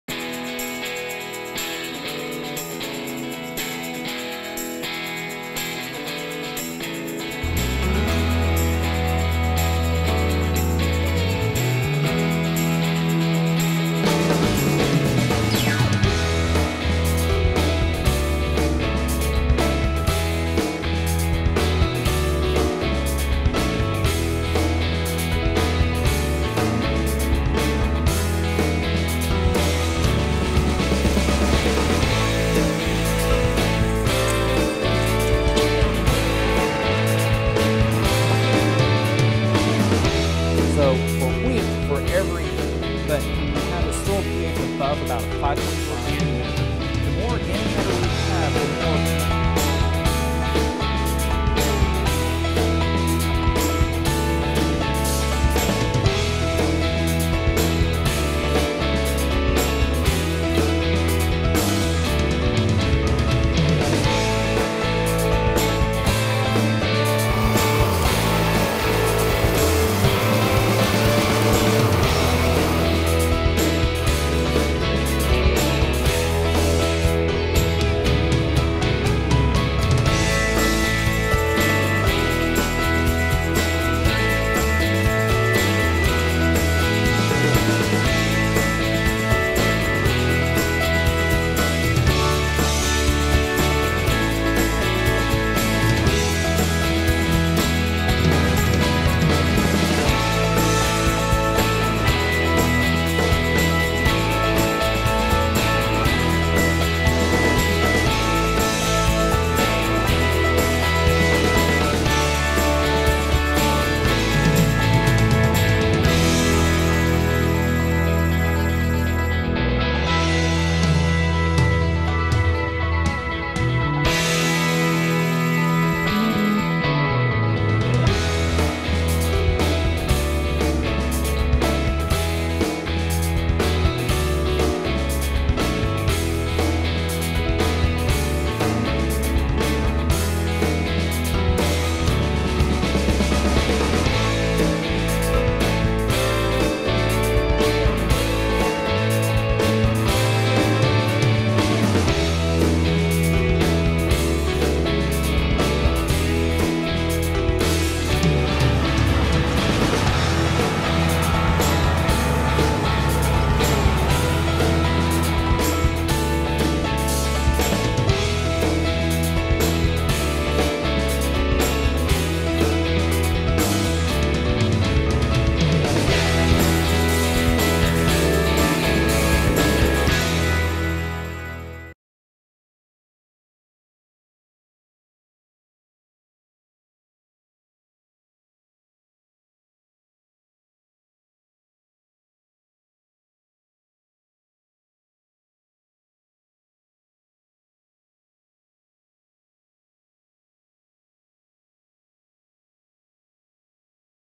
Oklahoma State and the Ohio State Universities 2018 InfoAg booth Video
This video highlighting the Oklahoma State and Ohio State Precision Agriculture programs was on display at the 2018 InfoAg conference, where both groups joined forces for a booth.